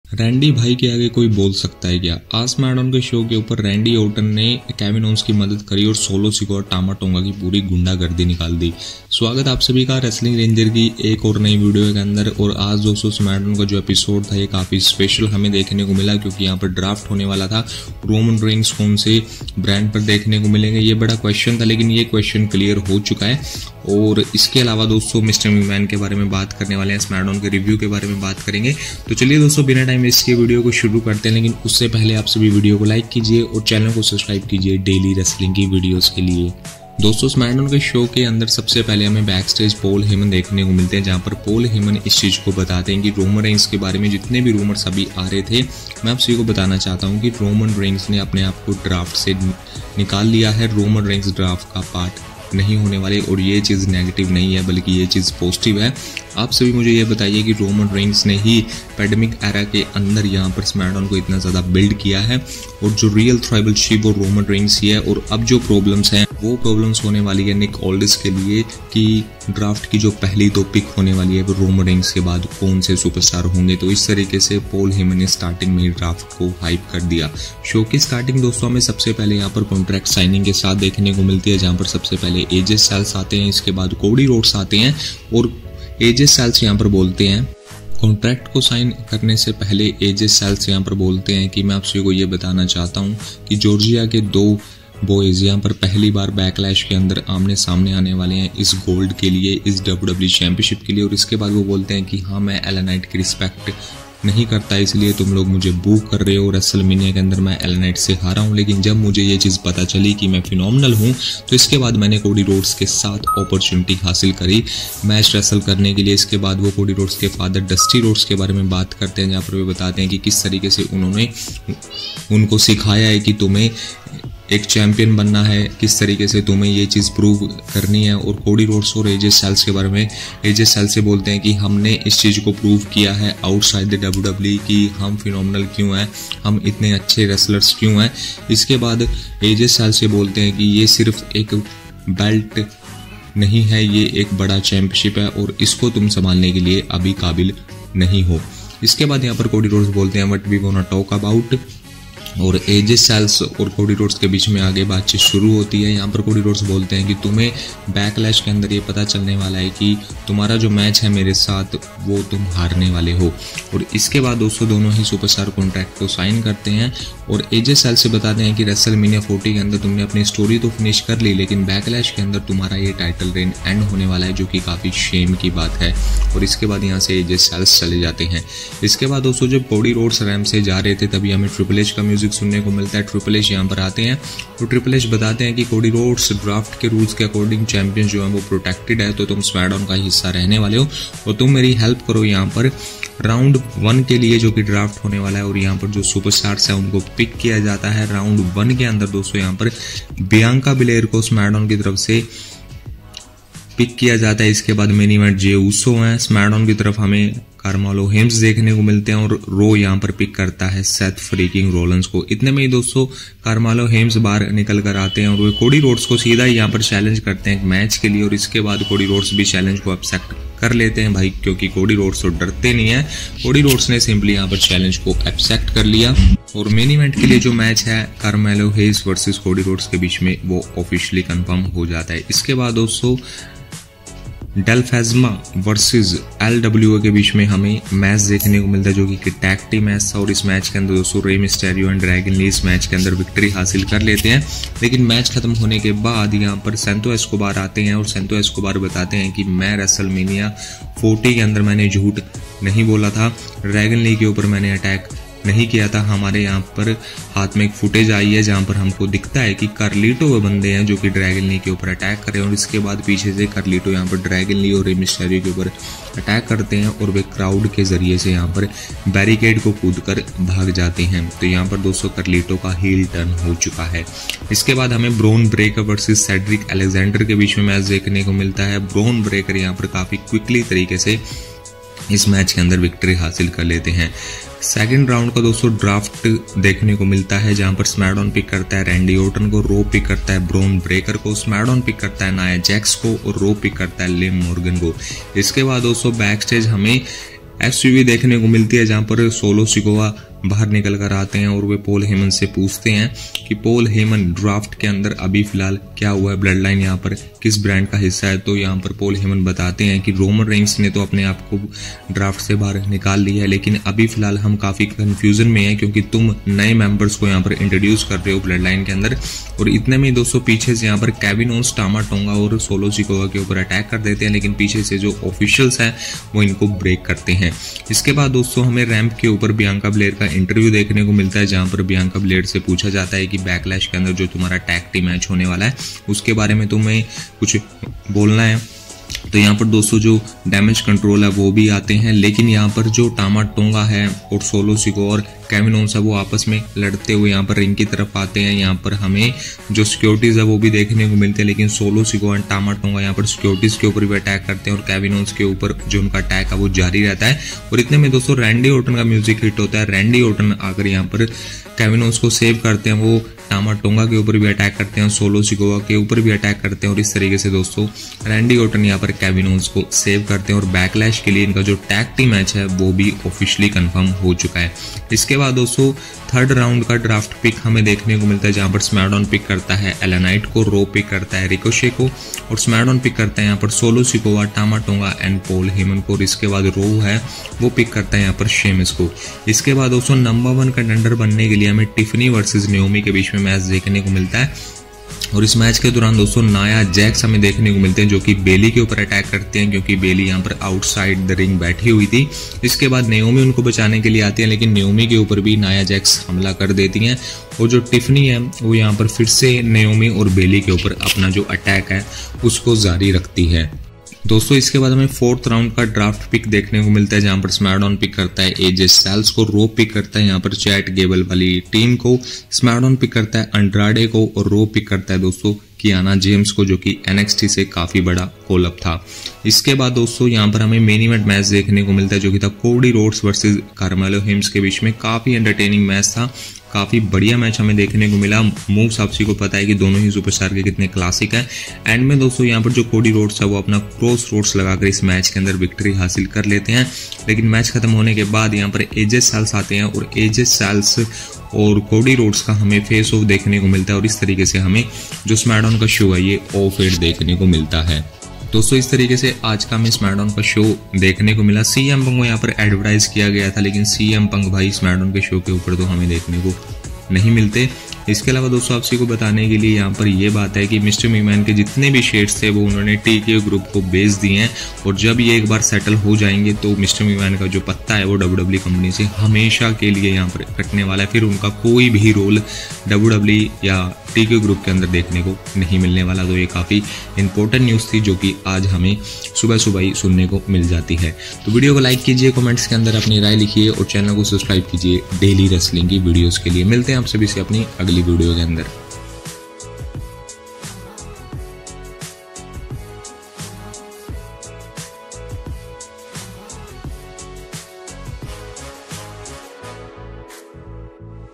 Vince McMahon Gone Forever - Randy Orton Attacks New Bloodline - WWE SmackDown 26 April 2024 REVIEW
WWE SmackDown Results – 4/26/24 (Night one of draft, Undisputed Championship contract signing)

About: Wrestling Ranger is created by Parveen Dhiman. This channel provides you with content related to Pro Wrestling.